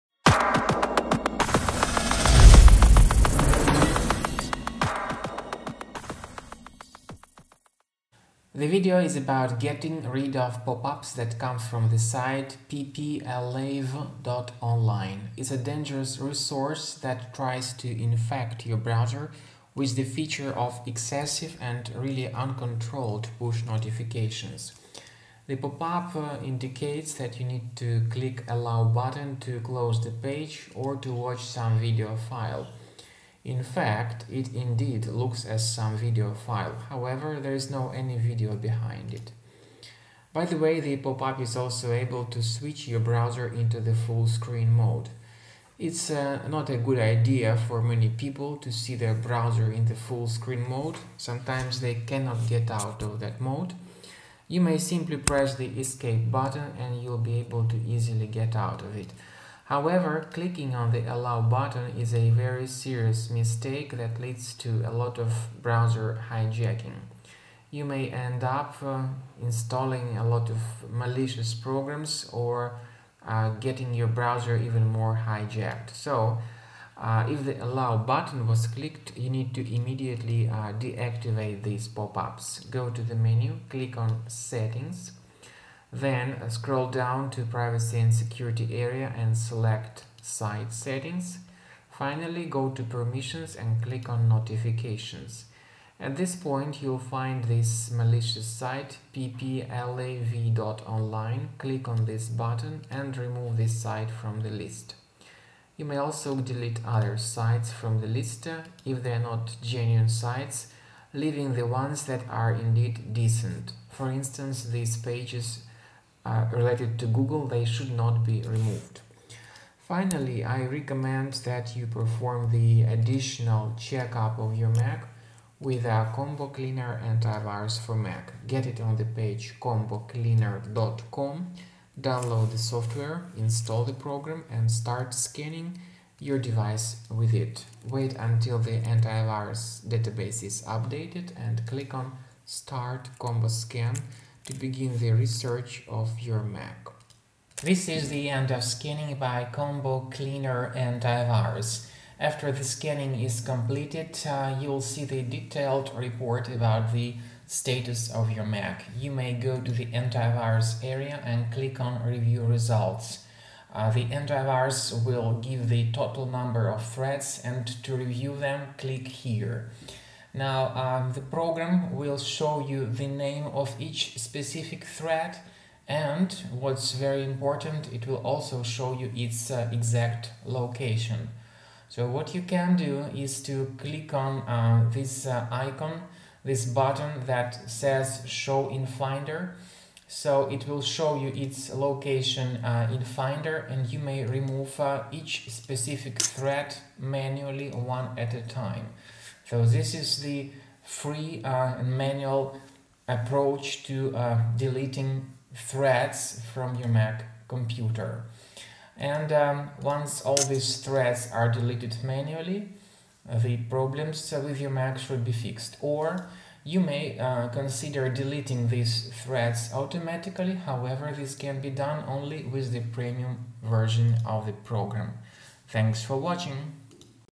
Ppalleyv.online pop-up ads - how to remove?
Ppalleyv.online is a malicious site that displays a lot of excessive web push notifications. Remove them with Combo Cleaner Antivirus or follow the manual recommendations in the video guide.